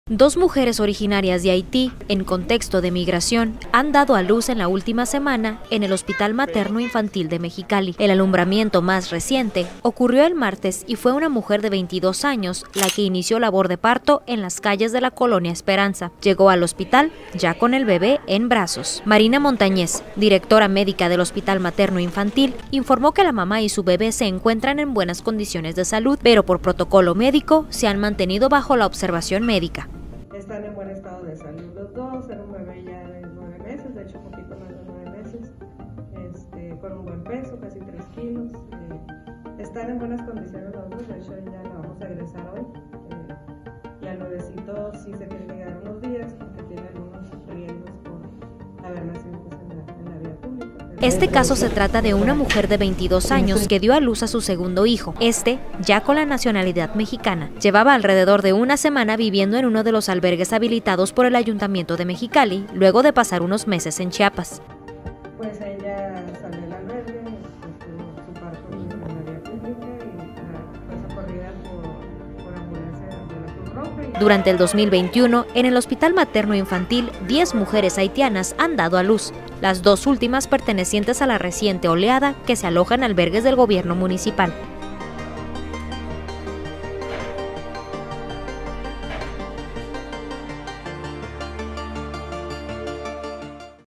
Dos migrantes haitianas han dado a luz en Mexicali | La Voz de la Frontera
Dos mujeres provenientes de las caravanas migrantes de Haití dan dado a luz a sus hijos en los últimos siete días en Mexicali, siendo tratadas en el Hospital Materno Infantil.

El nacimiento más reciente se sucitó la tarde del martes 21 de diciembre, en donde una mujer tuvo su labor de parto en la vía pública de la colonia Esperanza, por lo que llego al HMI con su bebé ya nacido.